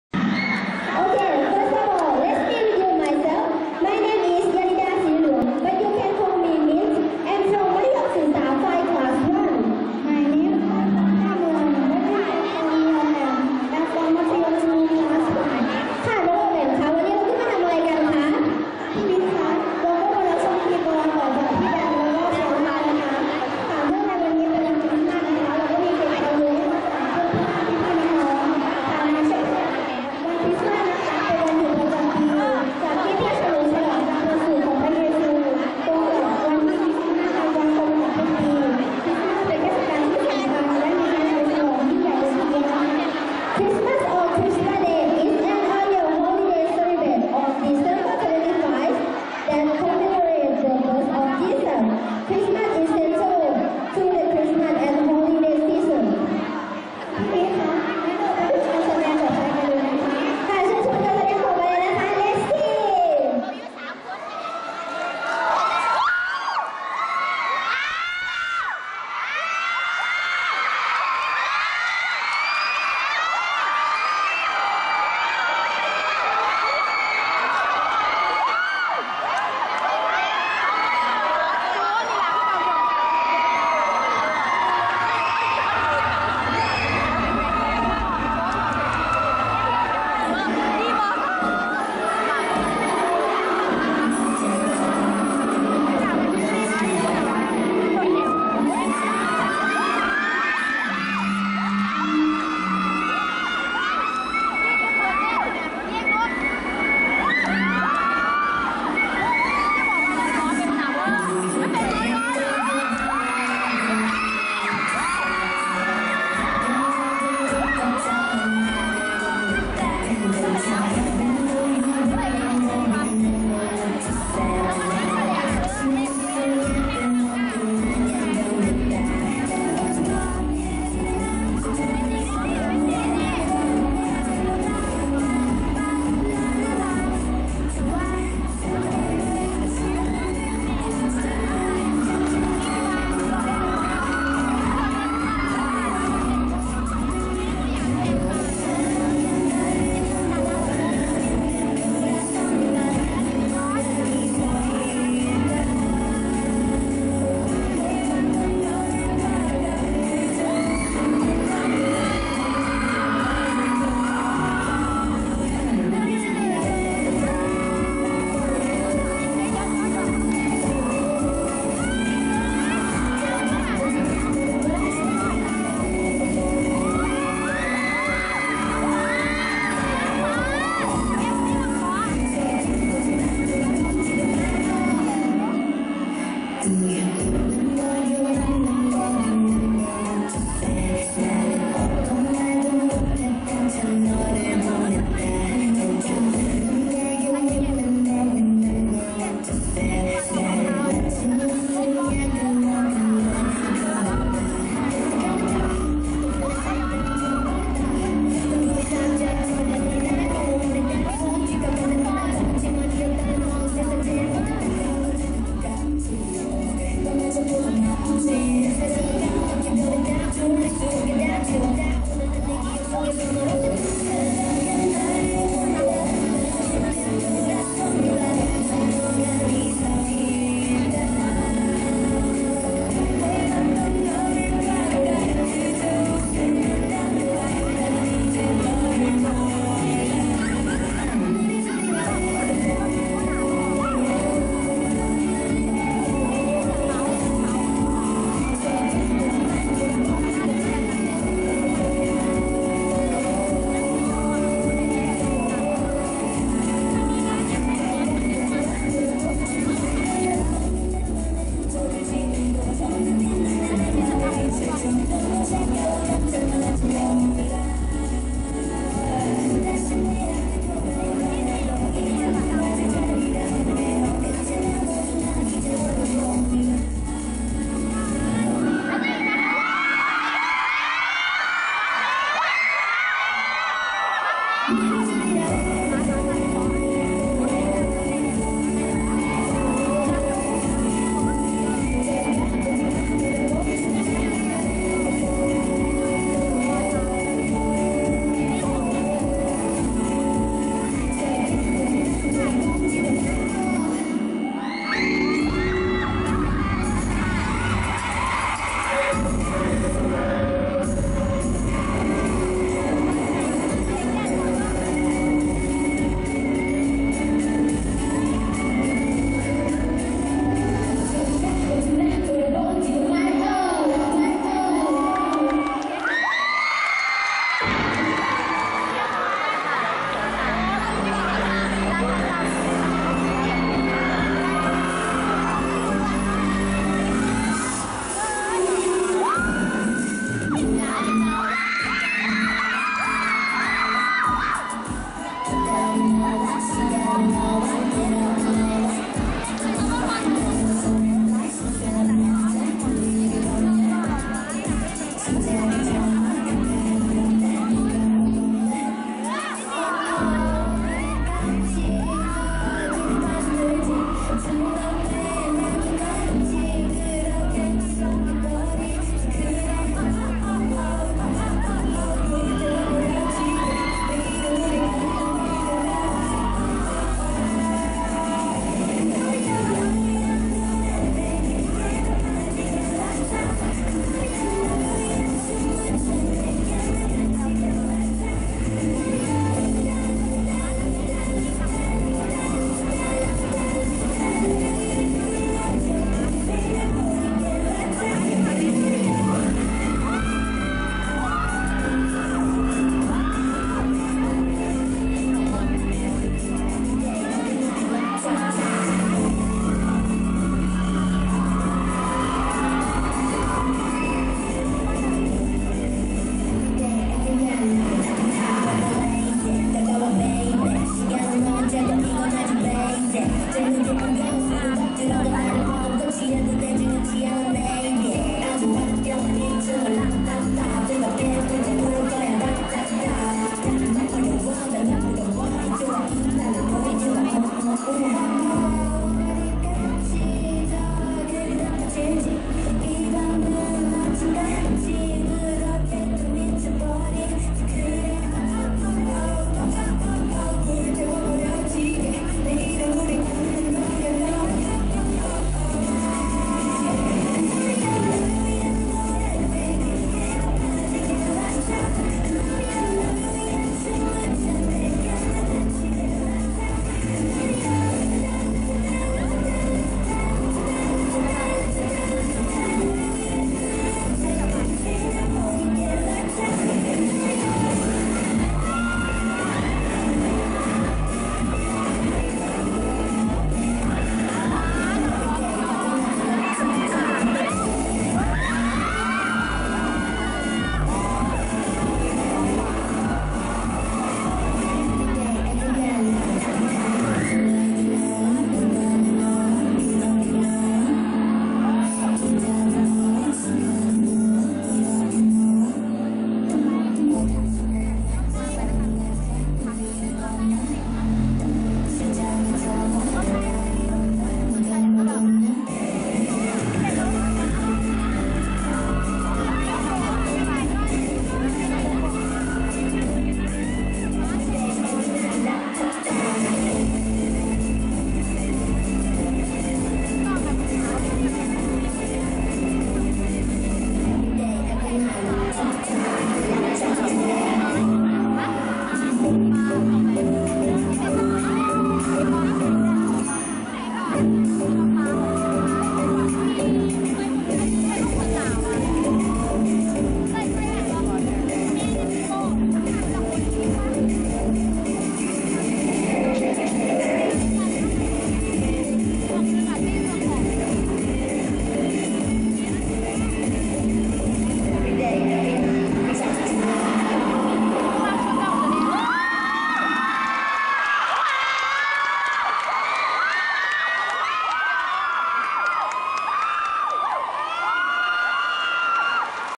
10 E P 10 Dancing ม 5 คริสต์มาส น พ ปี61
วันที่ 28 ธันวาคม 2561 นักเรียนชั้น ม.5 แสดง Dancing ในกิจกรรมวันคริสต์มาส  ณ  โรงเรียนนาะชือกพิทยาสรรค์ สพม.26